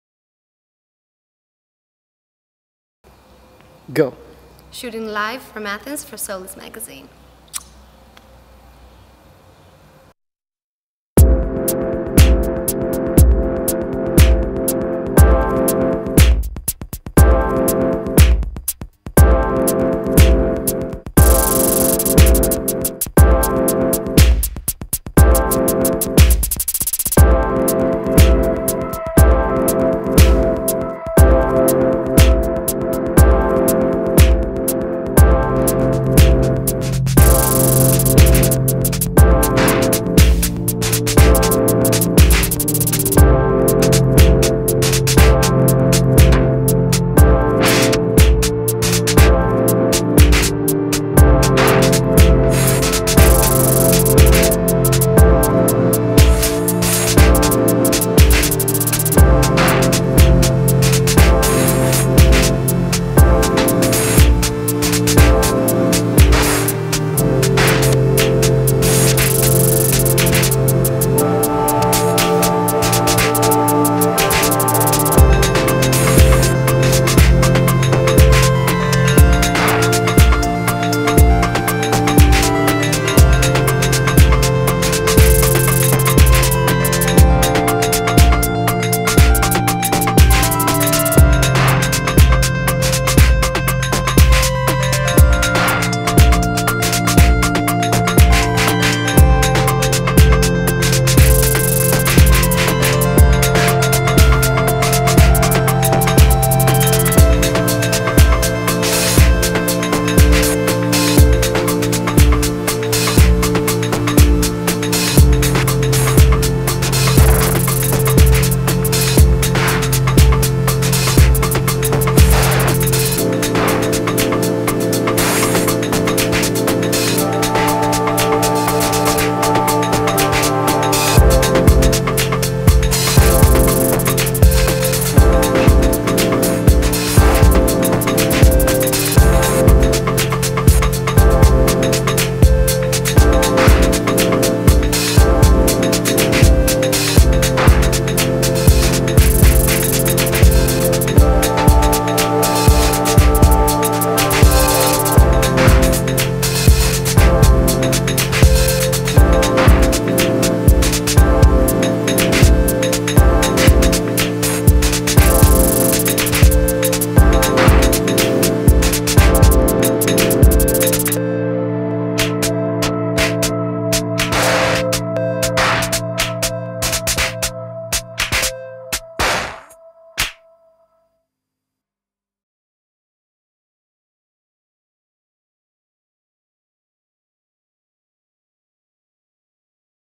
SOLIS INTERNATIONAL FALL 2014 - EVOLUTION FROM THE ASHES
Clothes: Celebrity Skin (Dimitris Strepkos) Production/styling: Giselle Karounis MUA: Giselle Karounis
Hair: Vassilis Saroglou
Videographer: Alexander Koromilas
Backstage photographer: Demy Papanikou
Photography assistant: Francine Jasmine Chrysan